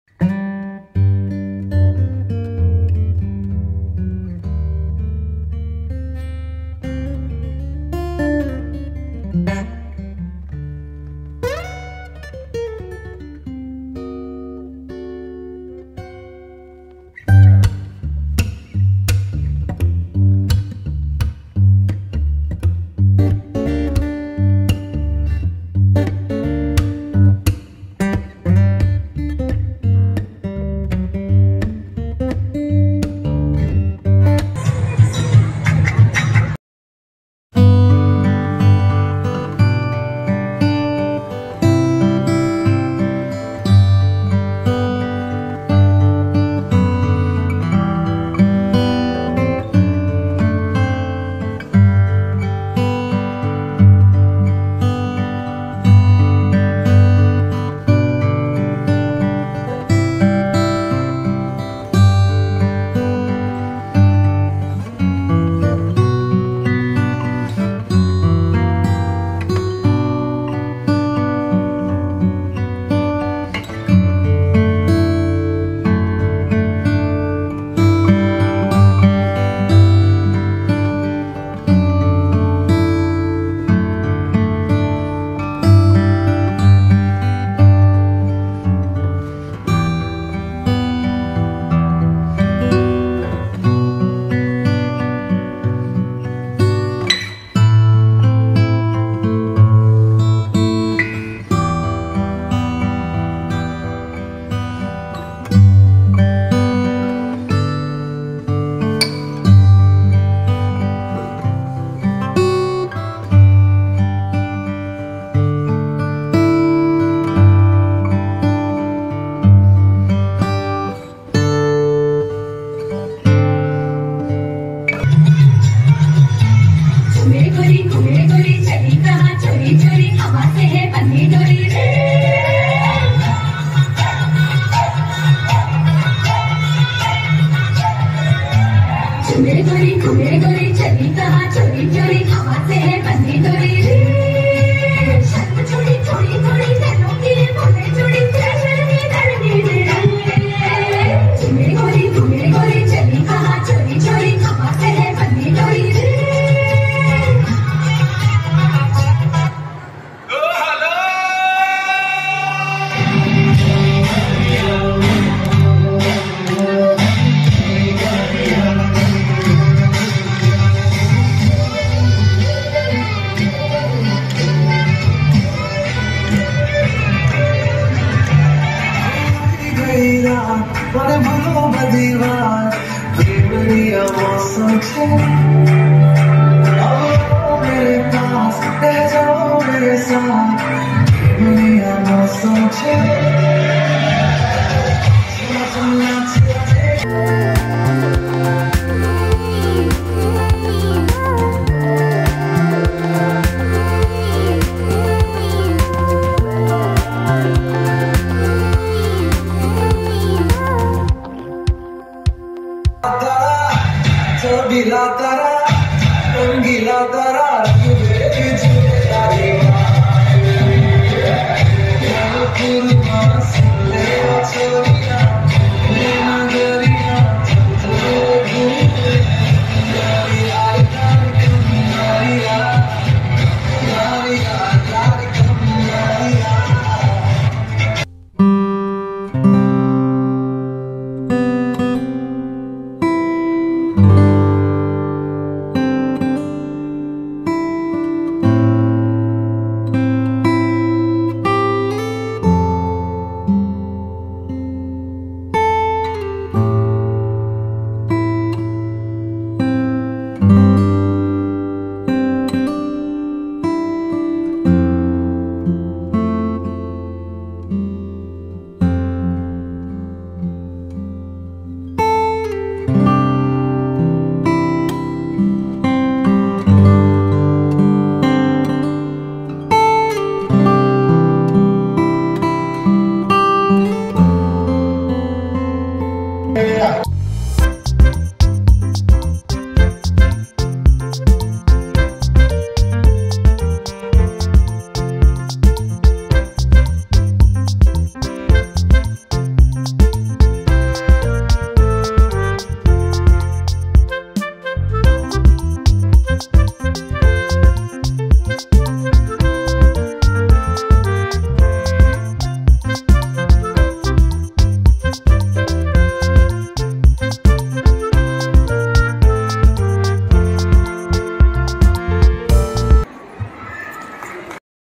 Excellent Performance by Blue Panther House
अलक्ष्या स्कूल के ब्लू पैंथर हाउस ने मचाई धूम